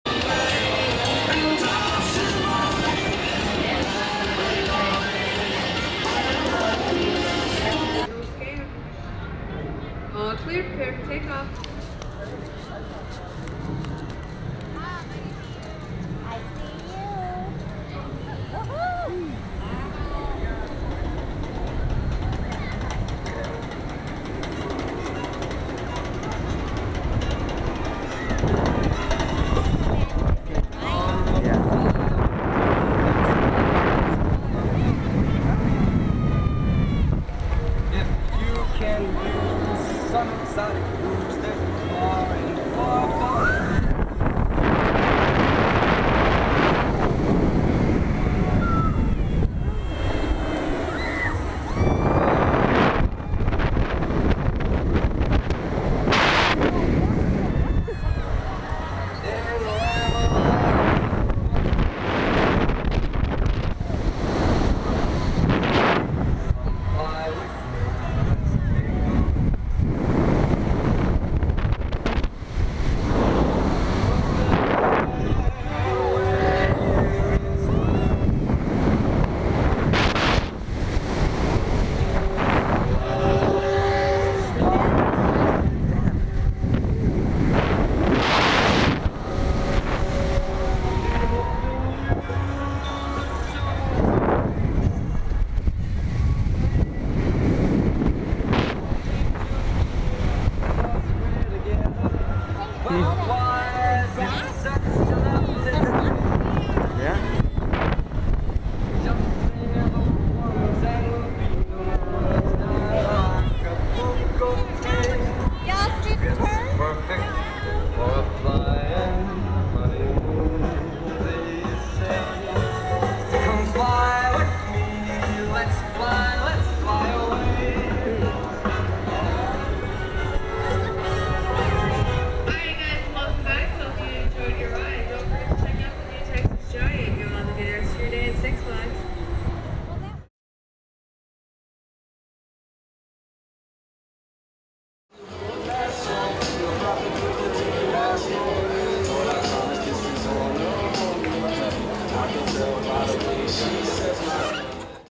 Texas Skyscreamer (HD POV) Six Flags Over Texas
Equipment I use:
-POV Camera
-Offride Camera: Sony HDR-CX550v
-Editing Computer
-Editing Software

Towering an astounding 400-feet above the ground, the Texas SkyScreamer will be the tallest swing ride in the world that only looks sweet and innocent. Once you are strapped into the open-air seat of this extreme swing ride, prepare for takeoff. The Texas SkyScreamer will start to slowly swing you around and around as it takes you all the way up to the very top of the tower where you'll spin around and around just below a gigantic Texas Lone Star. Before you know it you'll be going faster and faster, soaring in a 124-foot-wide circle with the wind blowing in your face at 35 mile per hour and your feet dangling in sheer terror 400-feet above Six Flags Over Texas.

Word of advice: Don't look down. It will make you weak in the knees when you realize those aren't ants wandering about on the ground below. If you're brave enough to open your eyes, by day you'll have a breathtaking aerial view of the park down below and an amazing panoramic view of the surrounding areas. By night, you will be able to view the absolutely amazing nighttime views of downtown Dallas, downtown Fort Worth, and of course Cowboys Stadium and the Texas Rangers Ballpark here in Arlington, TX.